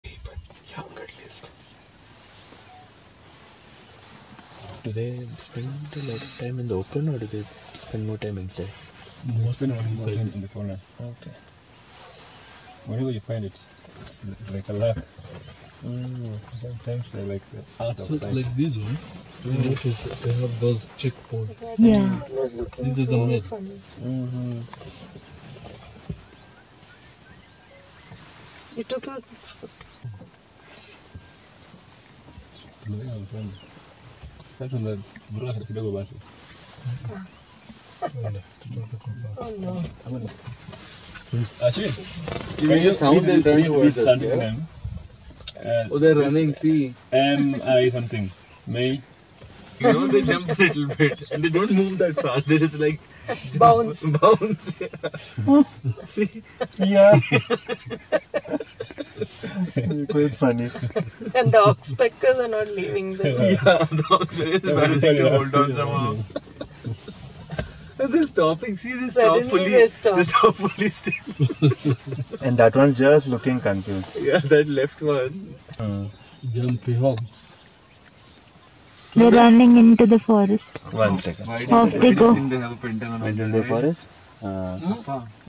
Giant Forest Hog at Aberdare NP
We saw a herd of this bizarre animal. They were so large that they even had Oxpeckers sitting on them.